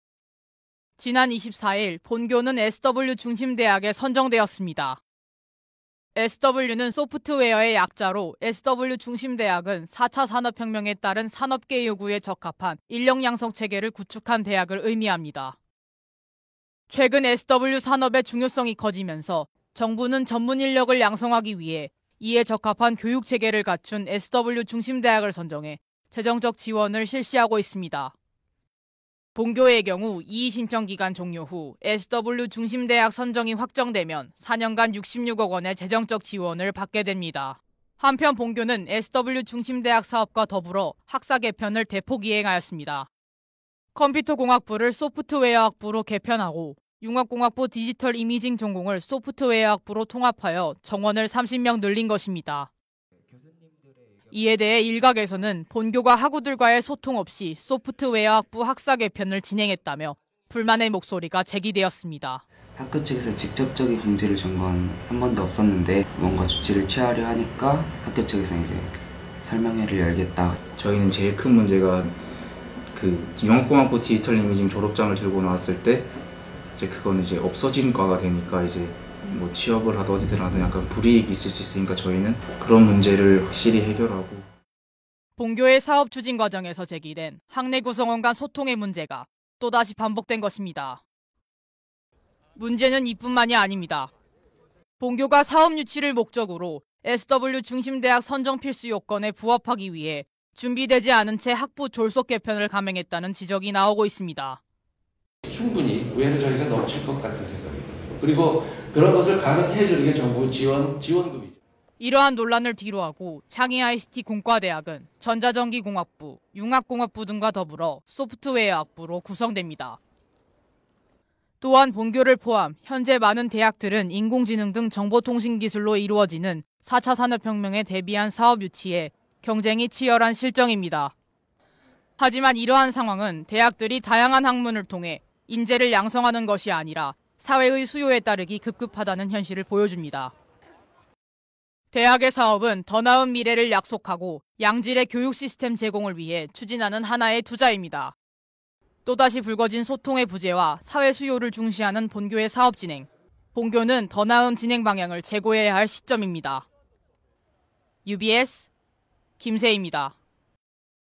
[UBS 영상뉴스] 본교 SW 중심대학 선정
지난 24일, 본교는 소프트웨어 중심대학으로 선정되었습니다. 하지만 본교의 사업 진행에 대해 소통의 부재, 사회수요를 중시한다는 의문이 제기되었습니다. 더 나은 본교의 사업 진행에 귀추가 주목됩니다.

 여기는 의와 참의 소리, 중앙대학교 방송국입니다.
UBS

보도 및 편집 / UBS 보도부